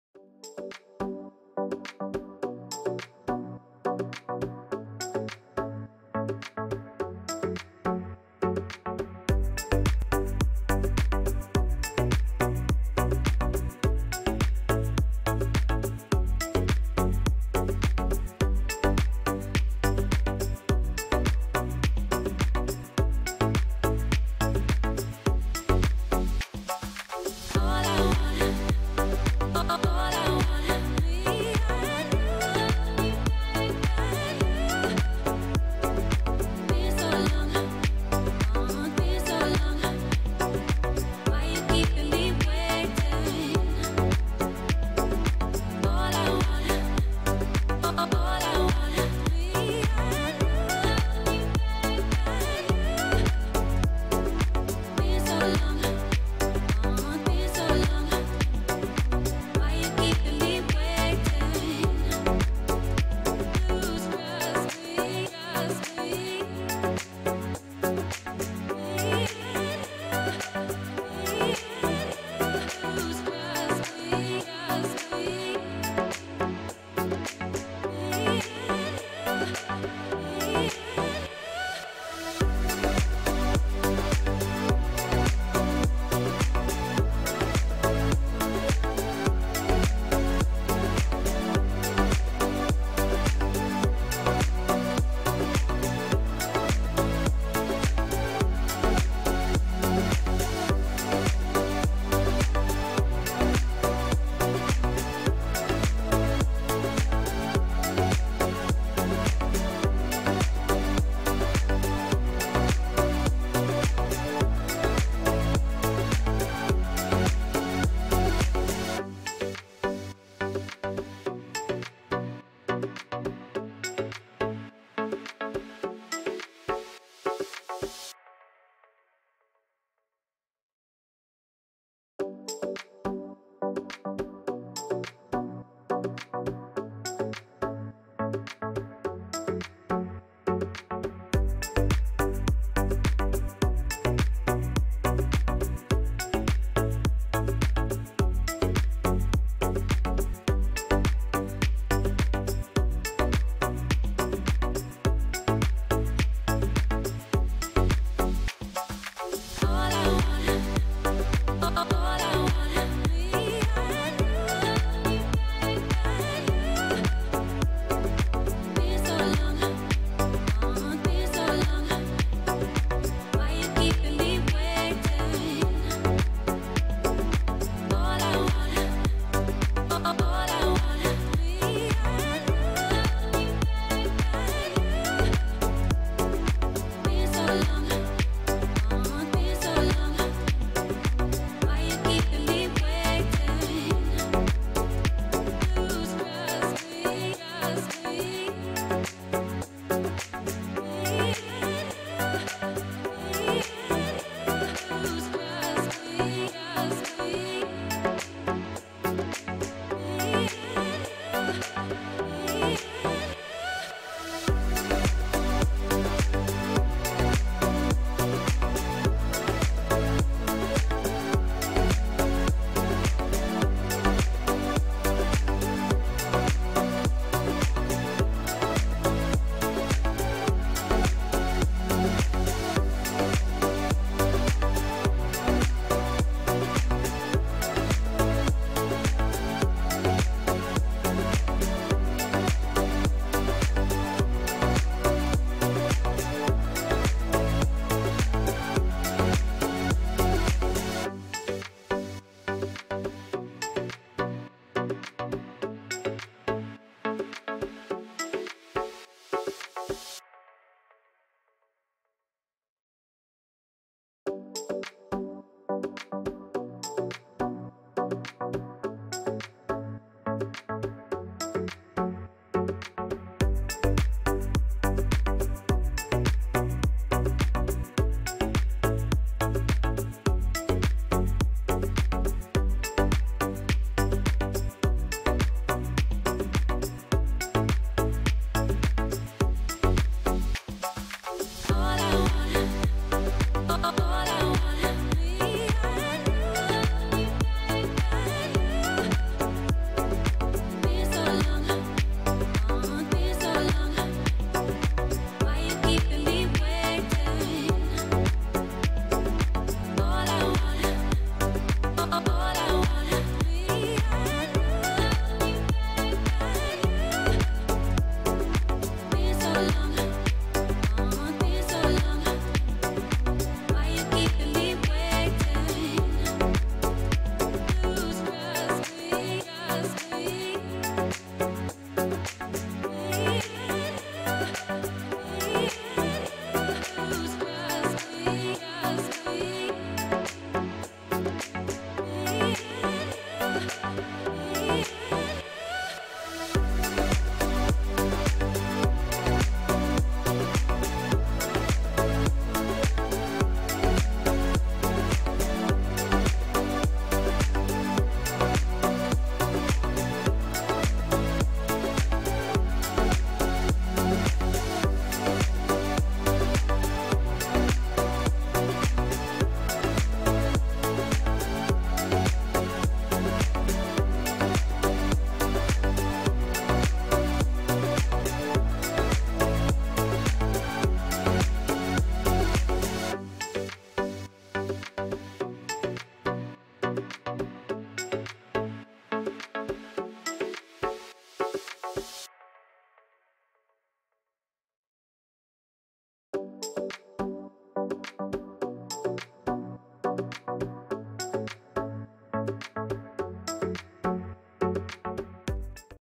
SAD SETH Garten of Banban 4 Easy Drawing & colouring pages for kids & adults
Fun colouring for kids SAD SETH Garten of Banban 4. Let's have fun with this Easy Drawing & colouring pages for kids & adults.